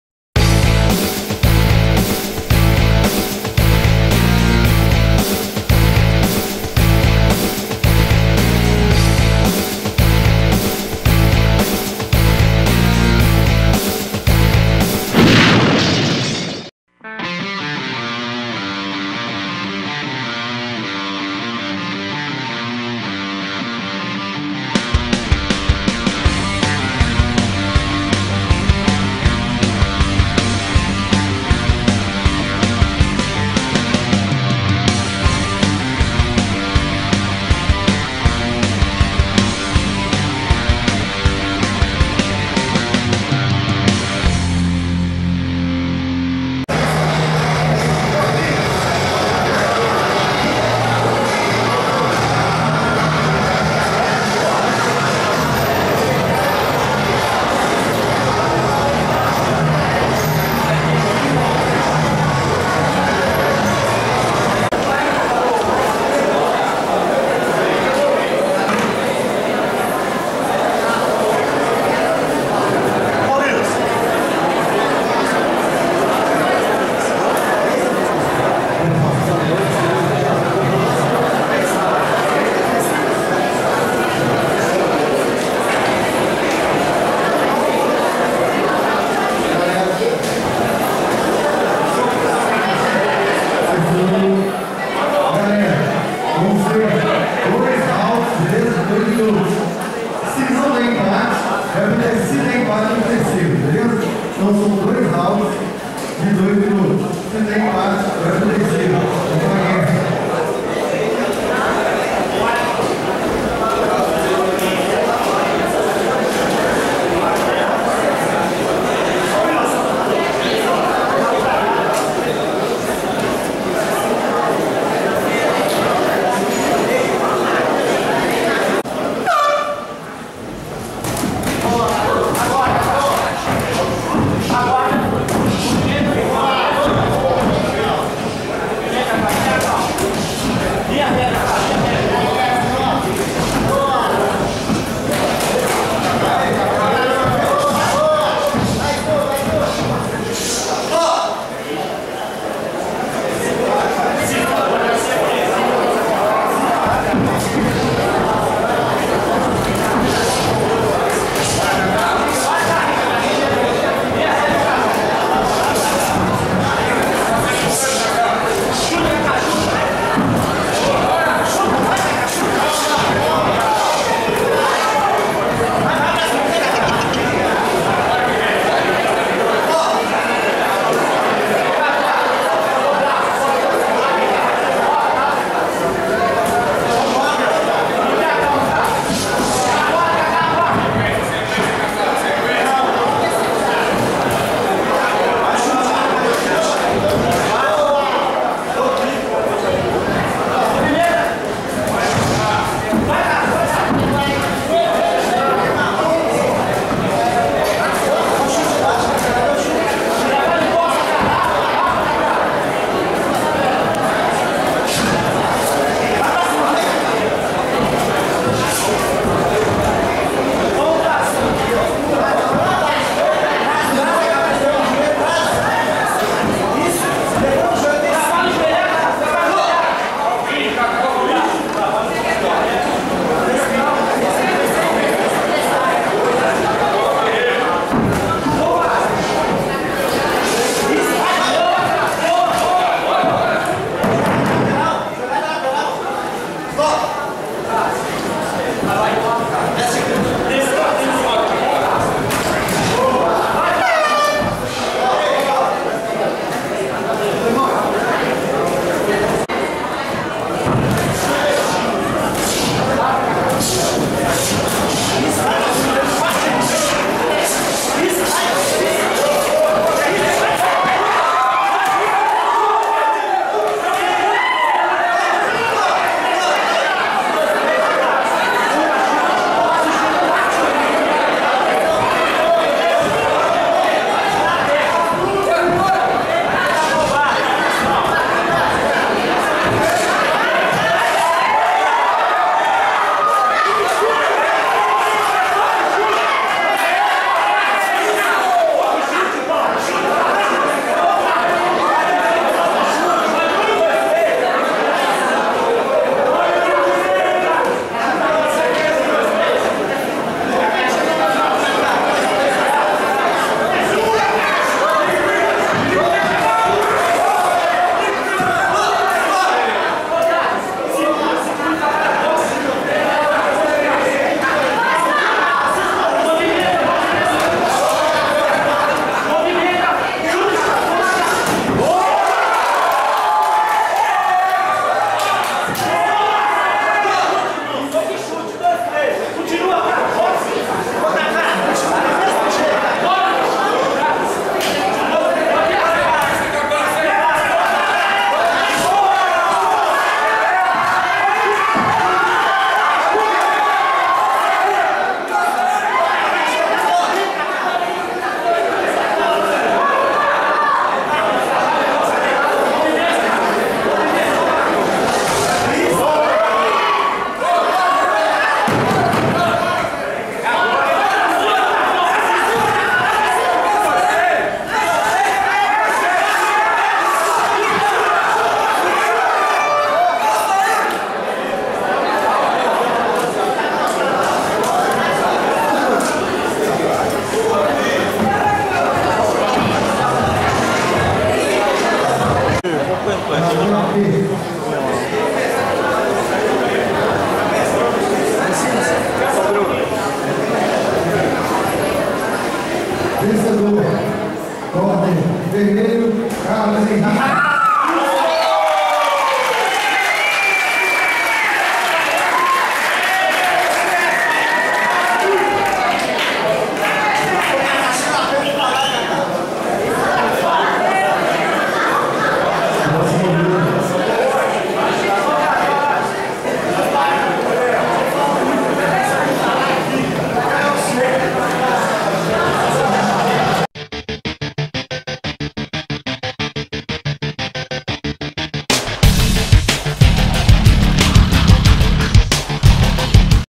Walex vs Carlos - BH Sparta 2
Carlos Henrique (Silva Fight Team) venceu por pontos Walex Michel (Pro Fight).